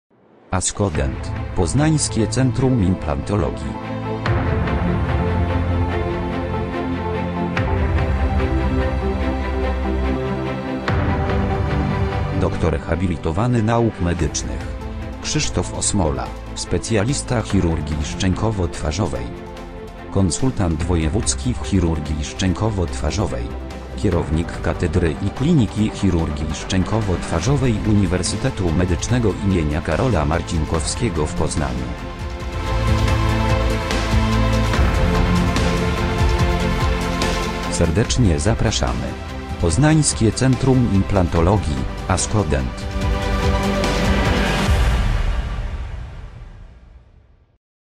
PCI ASKODENT - dr hab. n. med. Krzysztof Osmola
Prezentacja postaci jednego z założycieli Poznańskiego Centrum Implantologii ASKODENT - doktora Krzysztofa Osmoli.